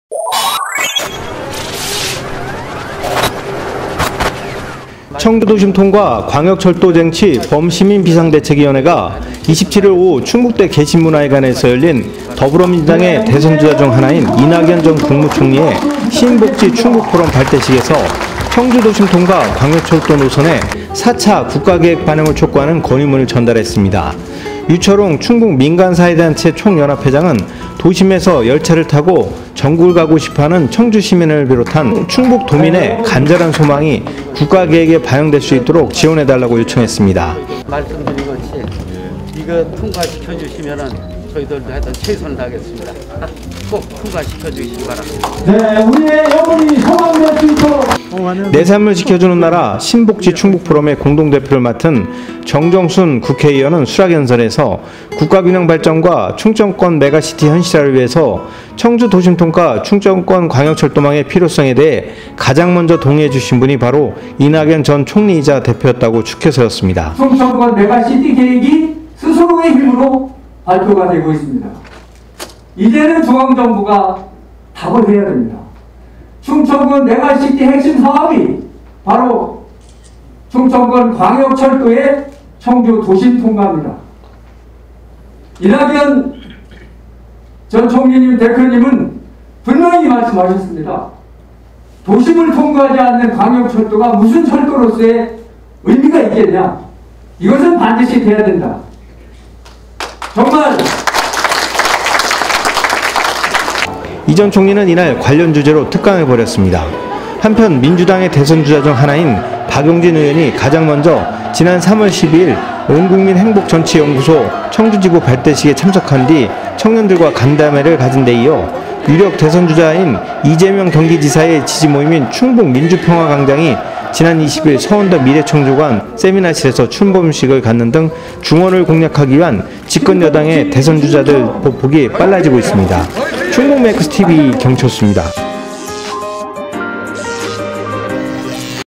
[cbMTV]범시민비대위, 청주도심통과 광역철도 반영 건의문 전달
이낙연 전 총리 신복지 충북포럼 발대식서
“충청권메가시티 필수사항 가장 먼저 약속”

[충북메이커스TV 경철수 기자]청주도심통과 광역철도 쟁취 범시민비상대책위원회가 27일 오후 충북대 개신문화회관에서 열린 더불어민주당의 대선주자 중 하나인 이낙연 전 국무총리의 신복지 충북포럼 발대식에서 청주도심통과 광역철도 노선의 4차 국가계획 반영을 촉구하는 건의문을 전달했다.
유철웅 충북민간사회단체총연합회장은 도심에서 열차를 타고 전국을 가고 싶어 하는 청주시민을 비롯한 충북도민의 간절한 소망이 국가계획에 반영될 수 있도록 지원해 달라고 요청했다.
'내 삶을 지켜주는 나라-신복지 충북포럼'의 공동대표를 맡은 정정순(더불어민주당·청주상당) 국회의원은 수락연설에서 "국가균형발전과 충청권메가시티 현실화를 위해선 청주도심통과 충청권광역철도망의 필요성에 대해 가장 먼저 동의해 주신 분이 바로 이낙연 전 총리이자 대표였다"고 추켜세웠다.
이 전 총리는 이날 관련 주제로 특강을 벌였다.
한편 민주당의 대선주자 중 하나인 박용진 의원이 가장먼저 지난 3월 12일 온국민행복정치연구소 청주지부 발대식에 참석한 뒤 청년들과 간담회를 가진데 이어 유력 대선주자인 이재명 경기지사의 지지모임인 충북민주평화광장이 지난 20일 서원대 미래창조관 세미나실에서 출범식을 갖는 등 중원을 공략하기 위한 집권 여당의 대선주자들 보폭이 빨라지고 있다.